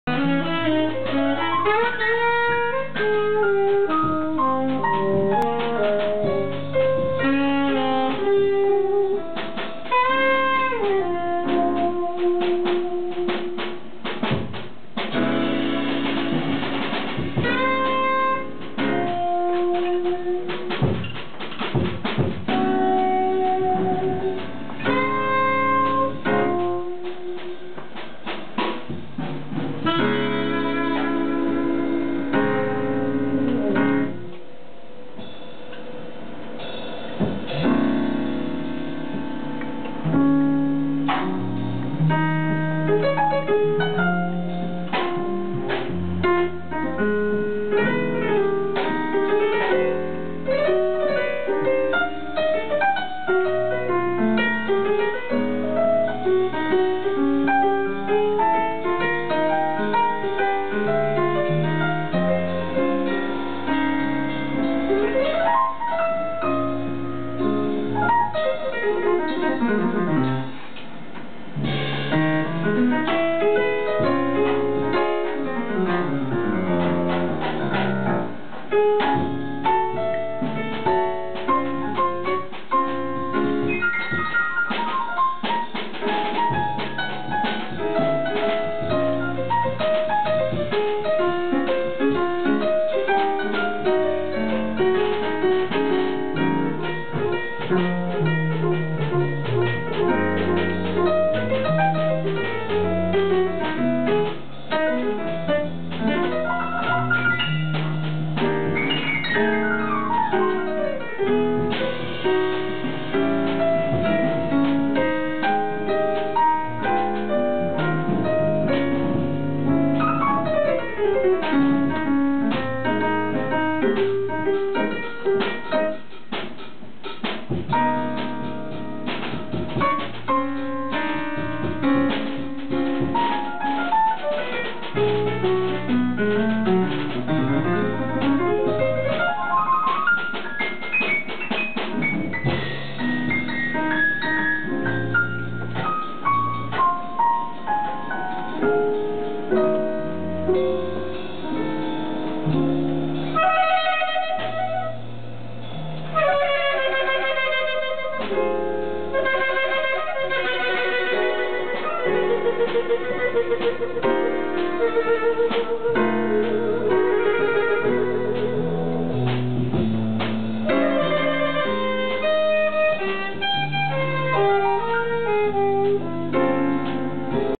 Hiroshi Minami at "Mame wo Maite"
Hiroshi Minami Live at "Mame wo Maite" Jazz-Club in Tokyos Daikanyama disctrict.